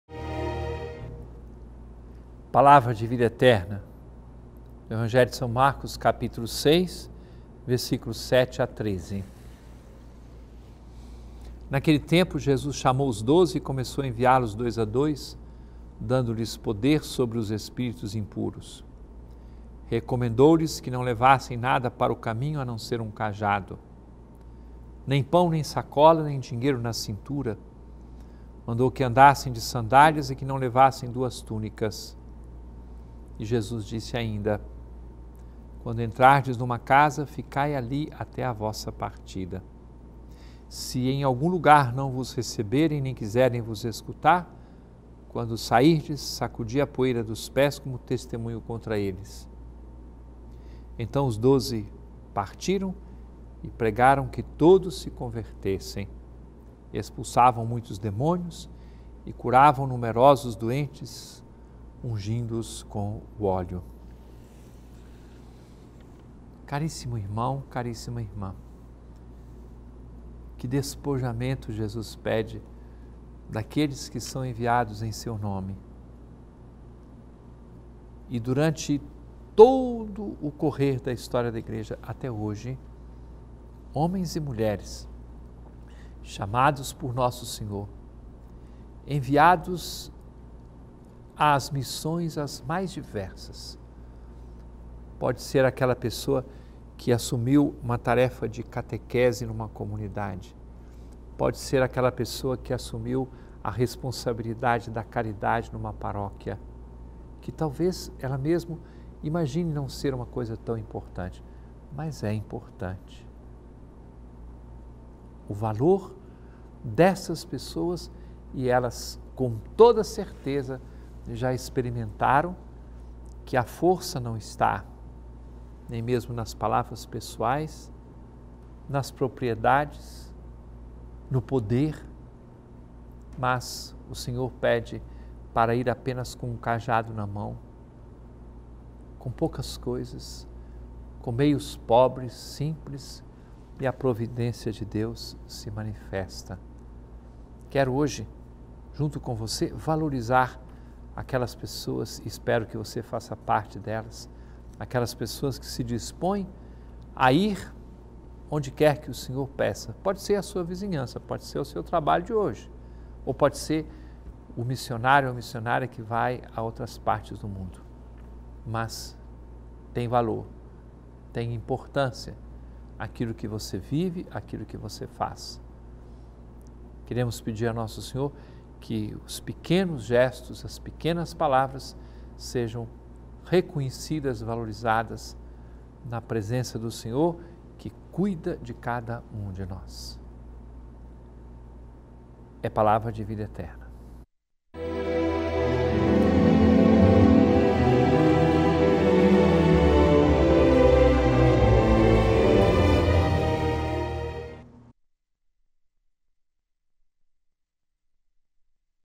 Palavra de Vida Eterna | São Marcos 6,7-13 - 05/02/2015
Naquele tempo, 7Jesus chamou os doze e começou a enviá-los dois a dois, dando-lhes poder sobre os espíritos impuros. 8Recomendou-lhes que não levassem nada para o caminho, a não ser um cajado; nem pão, nem sacola, nem dinheiro na cintura.

9Mandou que andassem de sandálias e que não levassem duas túnicas. 10E Jesus disse ainda: “Quando entrardes numa casa, ficai ali até vossa partida. 11Se em algum lugar não vos receberem, nem quiserem vos escutar, quando sairdes, sacudi a poeira dos pés, como testemunho contra eles!” 12Então os doze partiram e pregaram que todos se convertessem. 13Expulsavam muitos demônios e curavam numerosos doentes, ungindo-os com óleo.